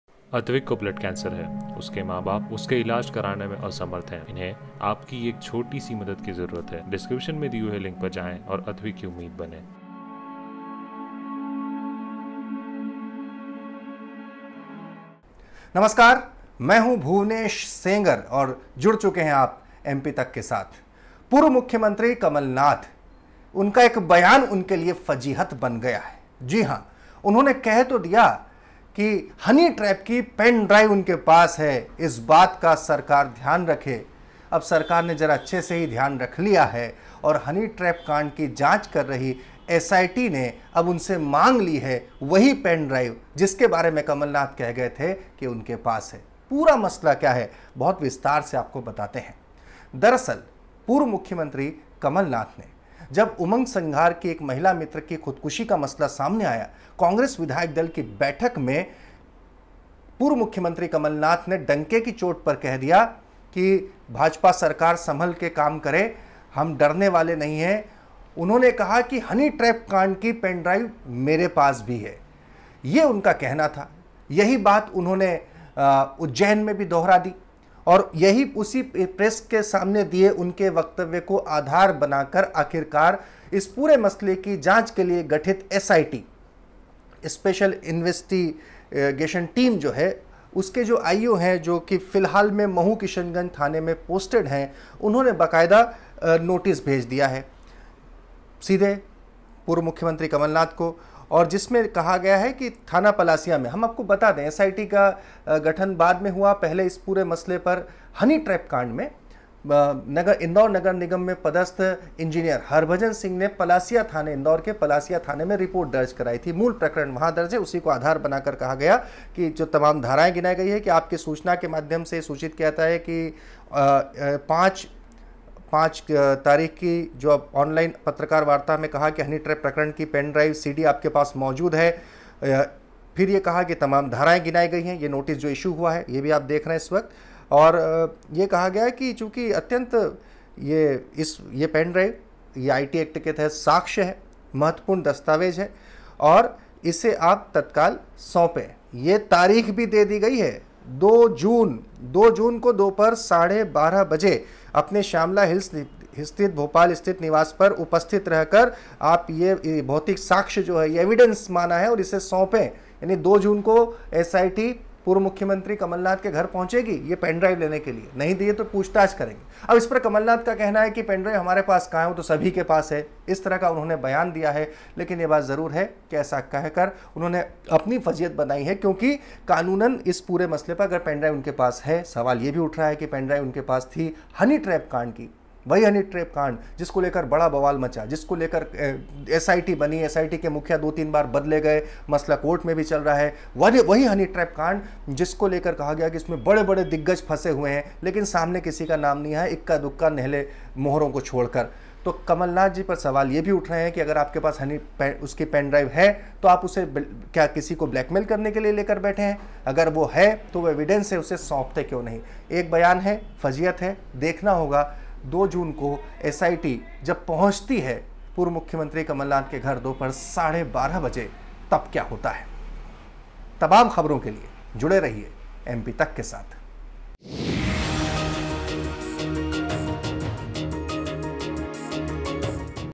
Honeytrap मामले को लेकर अब एमपी में आएगा सियासी भूचाल ! | MPTAK
एमपी के पूर्व सीएम कमलनाथ की मुश्किलें बढ़ गई हैं। पूर्व मंत्री उमंग सिंघार को फंसते देख कमलनाथ ने कहा था कि मेरे पास भी हनीट्रैप की पेन ड्राइव है। इसके बाद उनकी मुश्किलें बढ़ गई हैं। उनका इशारा था कि इस पेन ड्राइव में बीजेपी नेताओं के वीडियो हैं। वहीं, एमपी हनीट्रैप मामले की जांच कर रही एसआईटी ने कमलनाथ को नोटिस थमा दिया है।